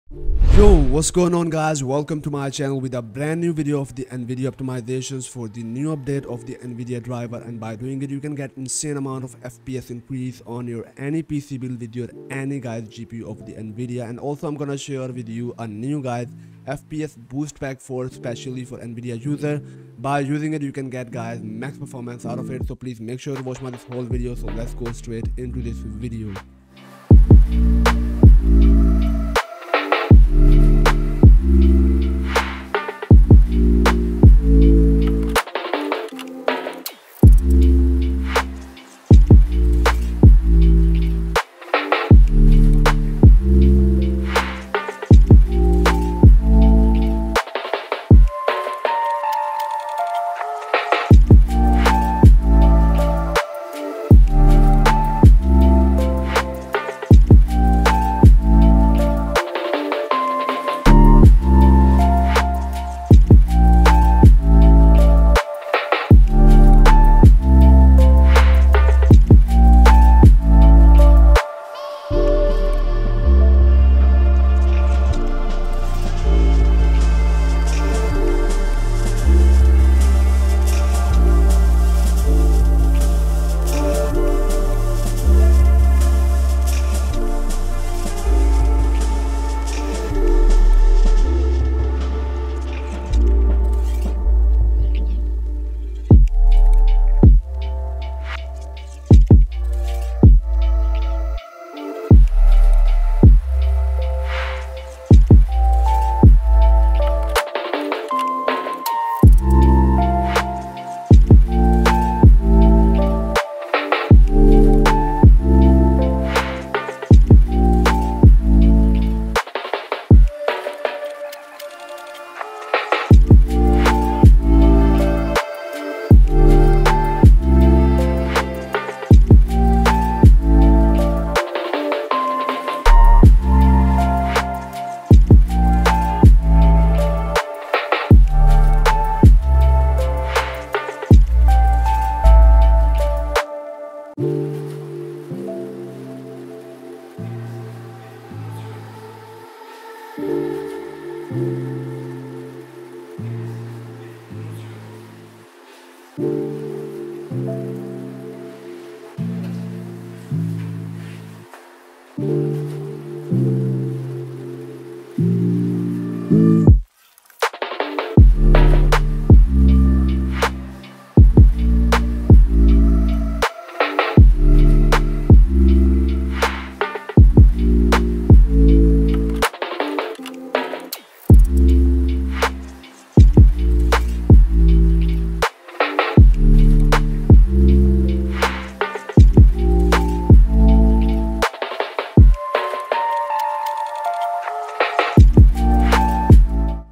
Nvidia Control Panel New Driver Update 511.79 (Gaming Optimization Guide 2022)
Hello, guys today in this video I'm going to show you about the new update of the NVIDIA and also I'm gonna show you the NVIDIA control panel's best settings for gaming by following this you can get insane amount of FPS increase in any PC.

►Title: Nvidia Control Panel New Driver Update 511.79 (Gaming Optimization Guide 2022)

►TIMELAPSE:

Intro (0:00)
Start Optimization (0:26)
Outro (3:17